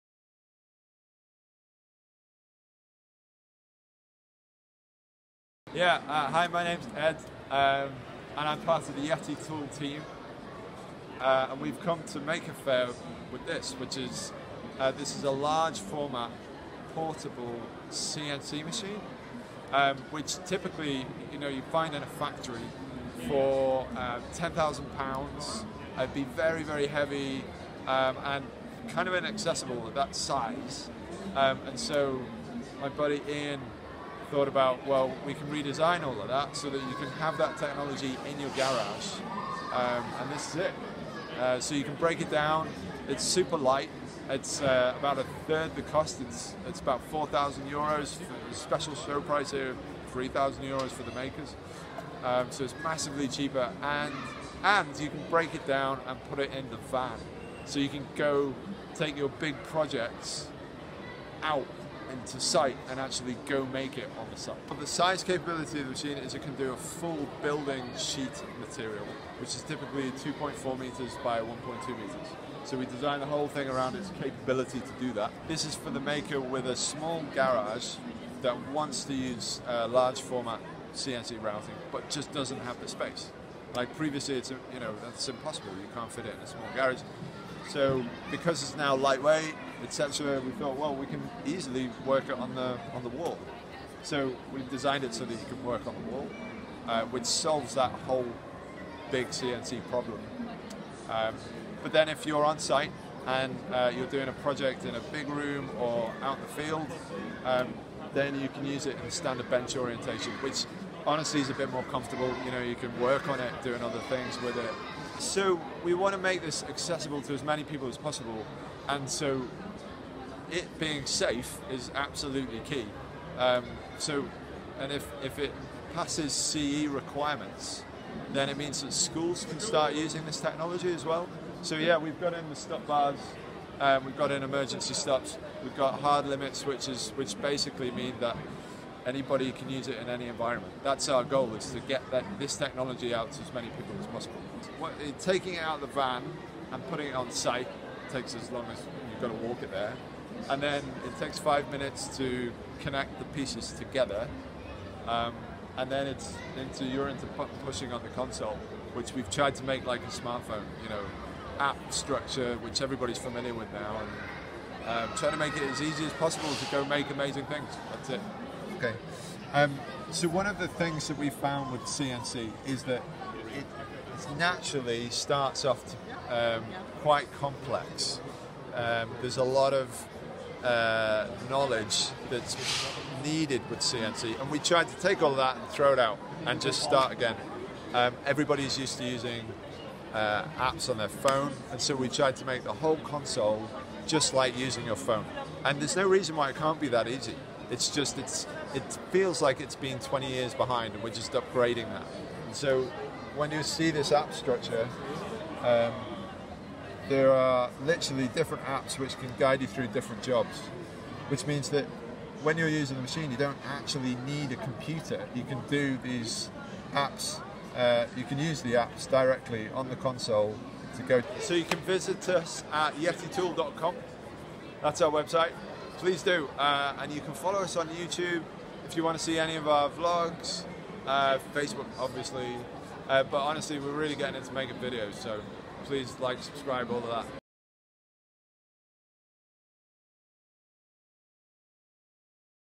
The Yeti Portable CNC Router
An interview with YETI about their full sheet portable CNC router from Maker Faire Paris